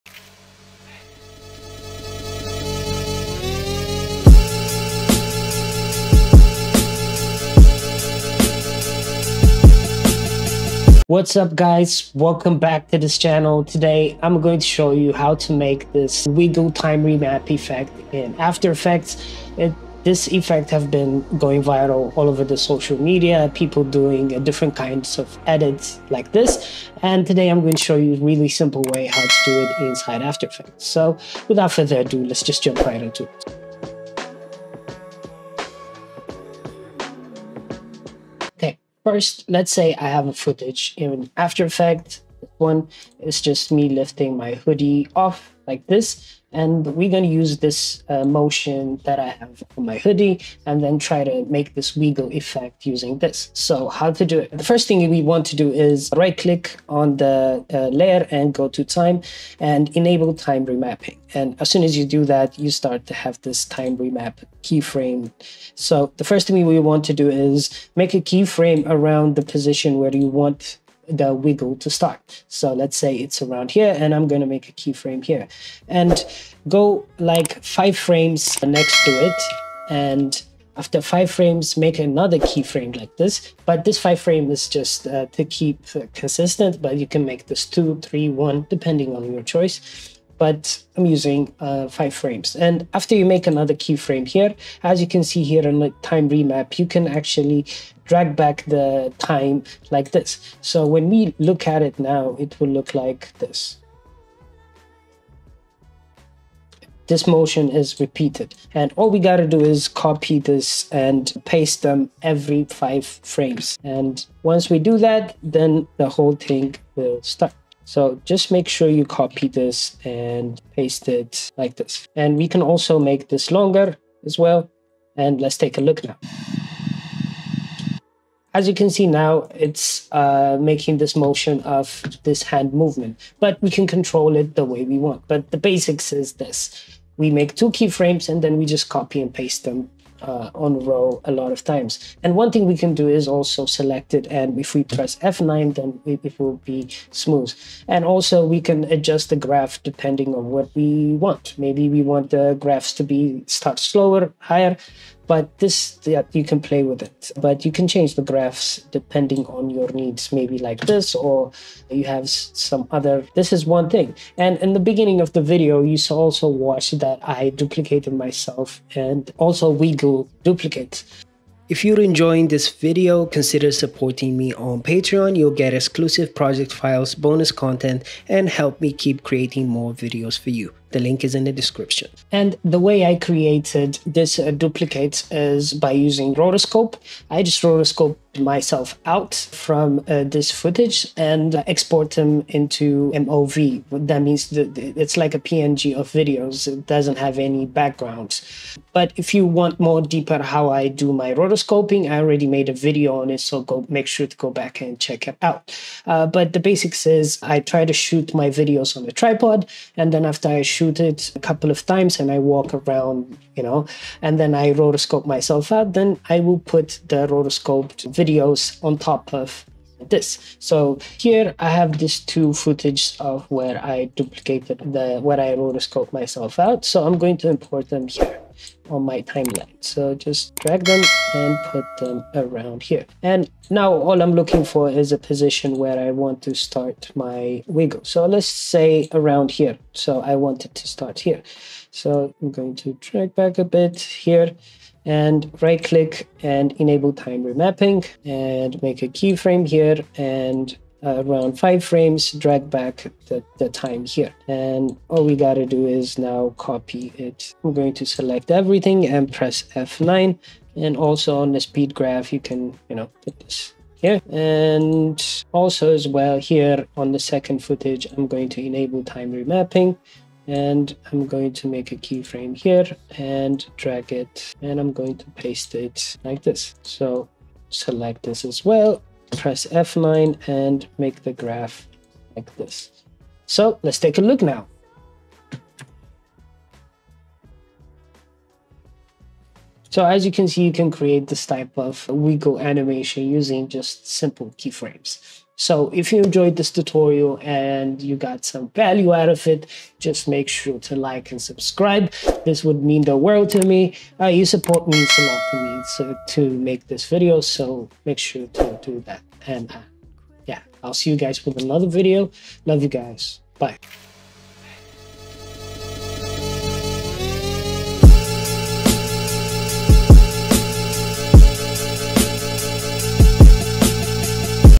How to Make the VIRAL Wiggle Time Remap Effect – After Effects Tutorial
Learn how to create the viral Wiggle Time Remap effect in Adobe After Effects! This trending effect is all over TikTok, Instagram Reels, and YouTube Shorts and in this step-by-step tutorial, I’ll show you exactly how to pull it off with ease. Whether you’re new to time remapping or want to add some extra motion and energy to your edits, this technique will take your videos to the next level. Perfect for music videos, reels, or creative transitions!

💬 Got questions or ideas?